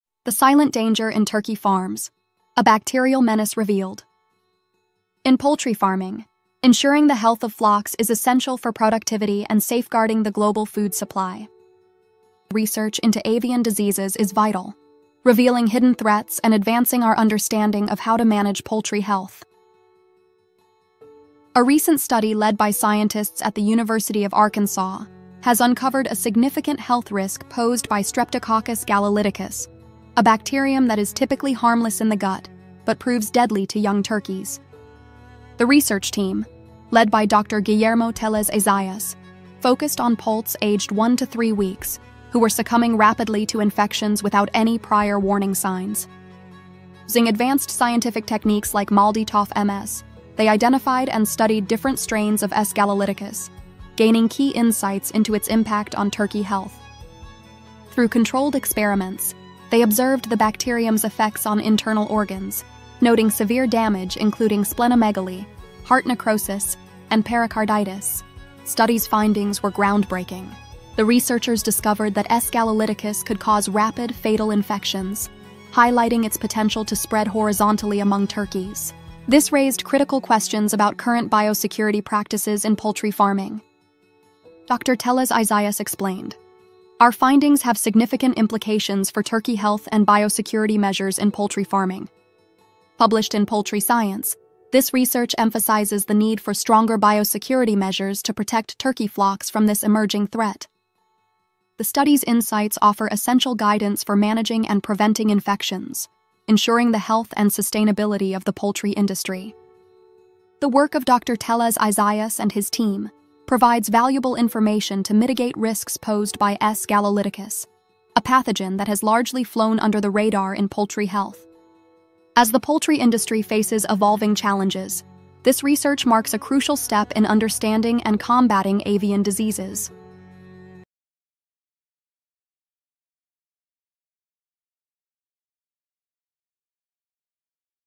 The Silent Danger in Turkey Farms: A Bacterial Menace Revealed
A study from the University of Arkansas reveals Streptococcus gallolyticus as a deadly threat to young turkeys, causing severe conditions like heart necrosis and splenomegaly. The findings emphasize its rapid spread and the need for stricter biosecurity in poultry farming.